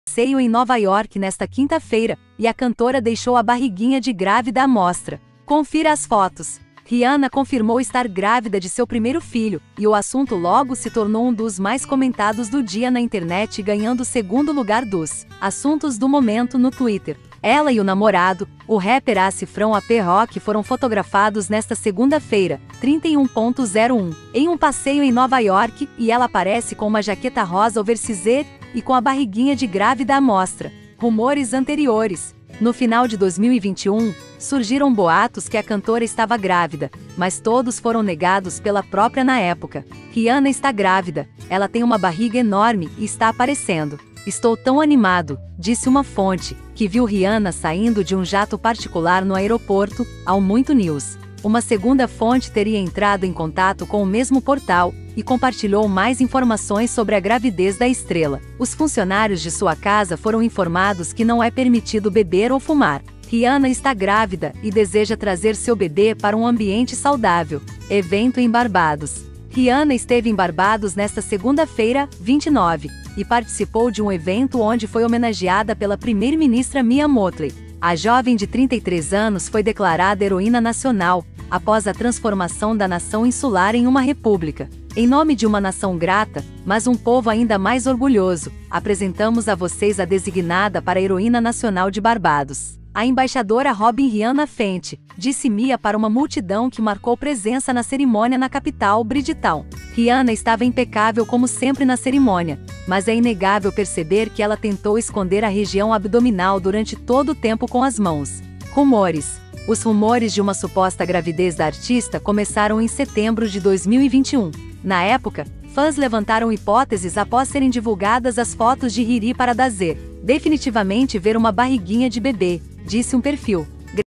Você sabia? RIHANNA ESTÁ GRÁVIDA DO SEU PRIMEIRO FILHA 🤰
A cantora RIHANNA ESTÁ GRÁVIDA DO SEU PRIMEIRO FILHA 🤰